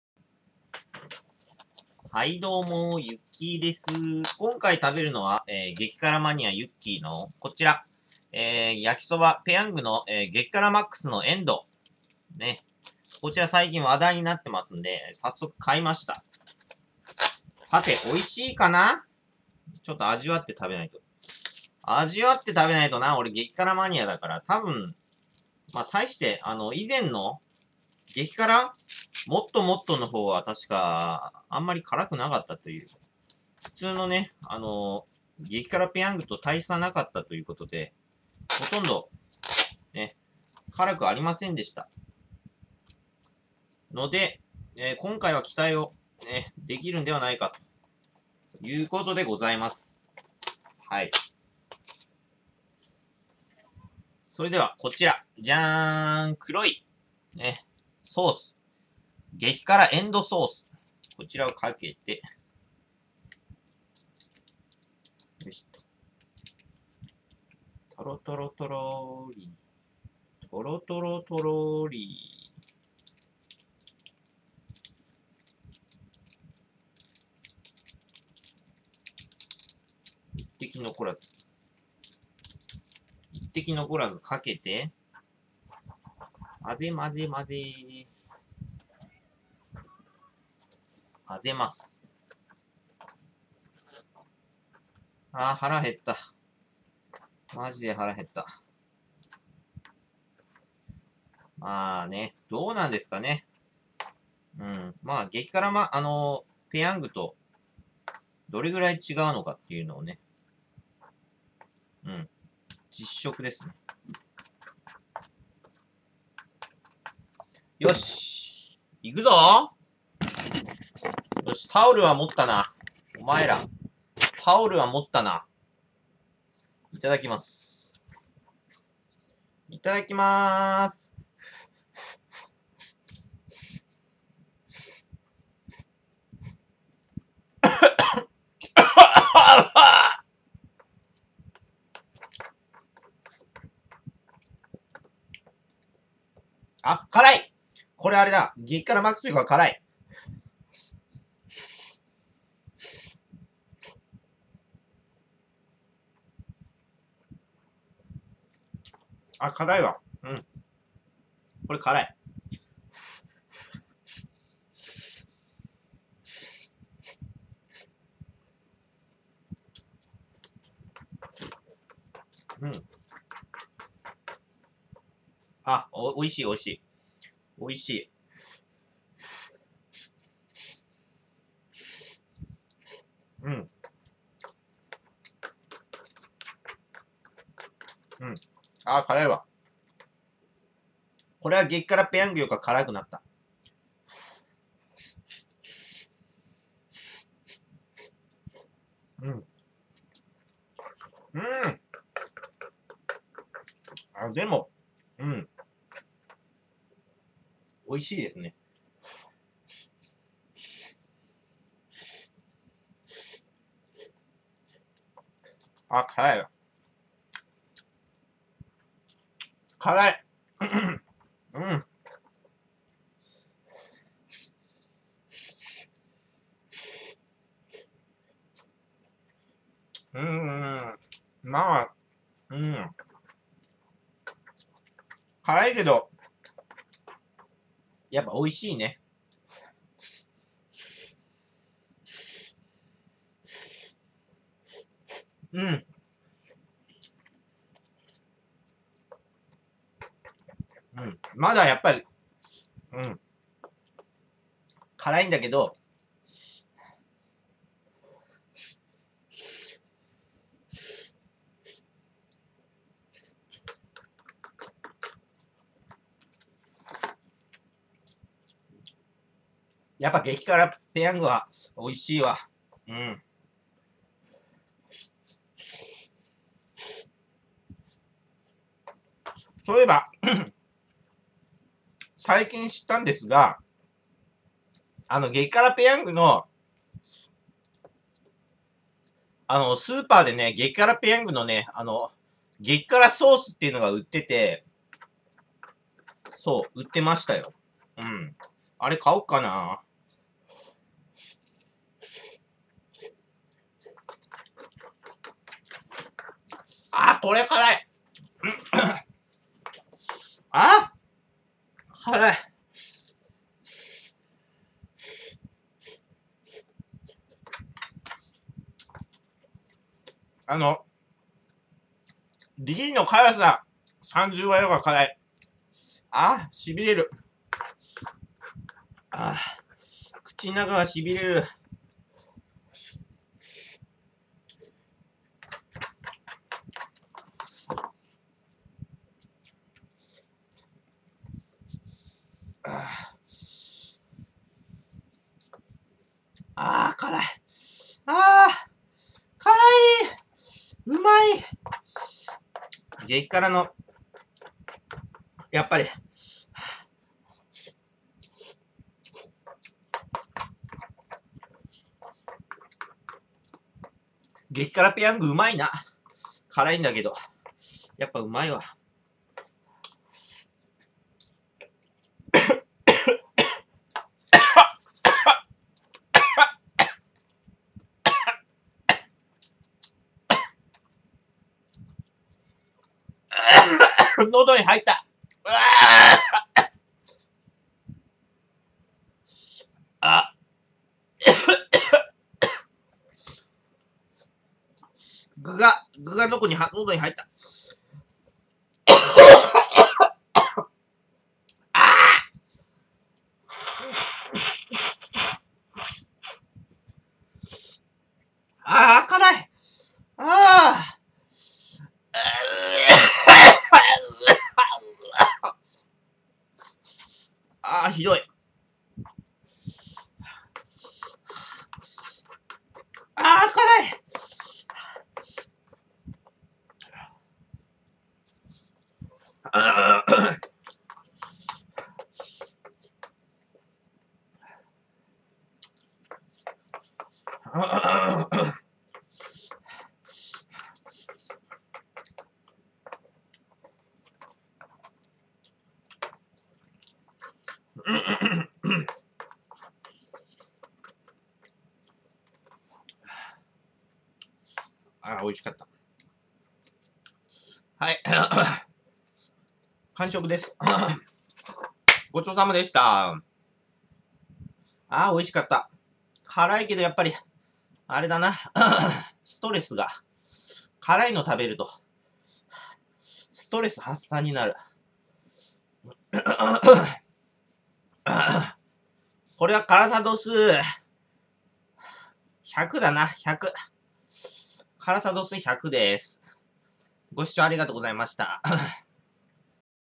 激辛マニアユッキーが「ペヤング激辛MAX END」を食う！ 【激辛】
激辛度数：１００

Twitterが新しくなりました⇒@yukkituikyasu